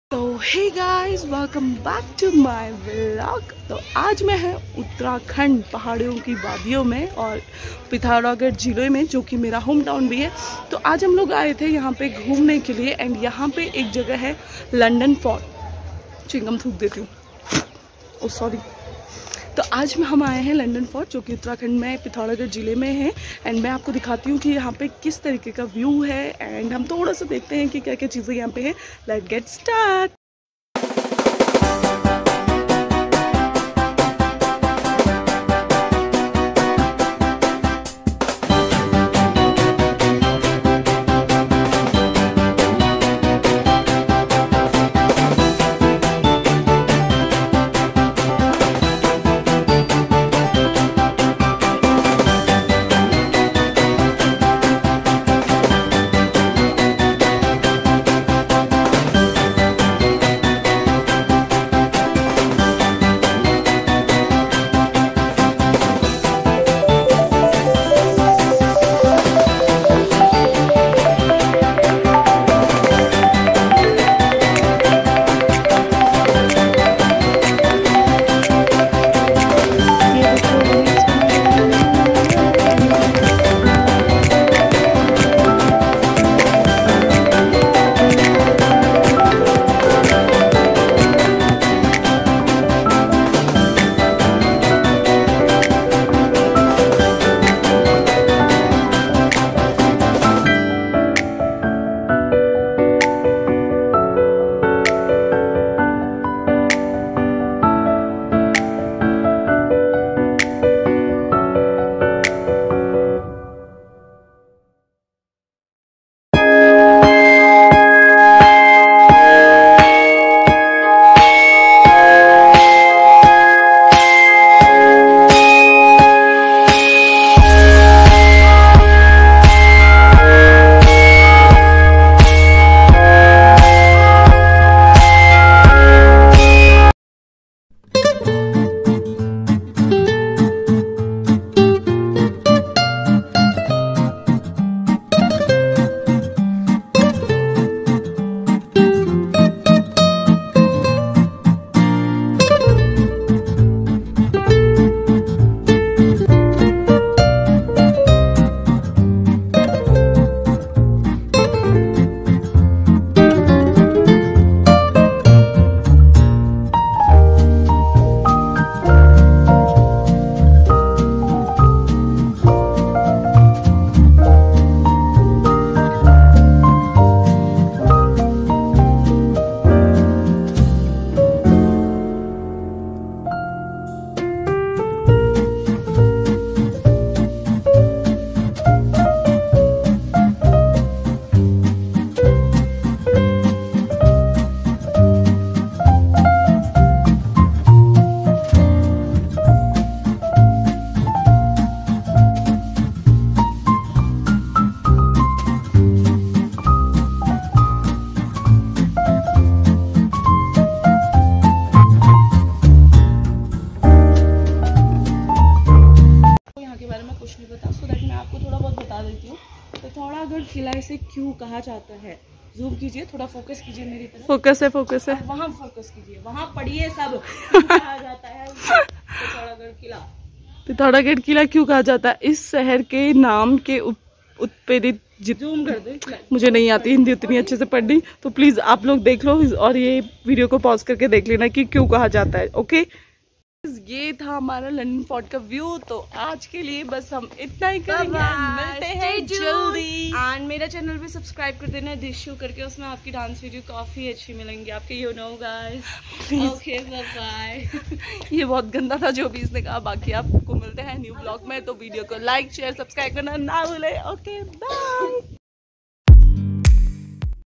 London fort || pithoragarh || uttrakhand ||full fun ||Himalayan view 😍😍😍😍😍
london fort view very nice in pithoragarh, utrakhand .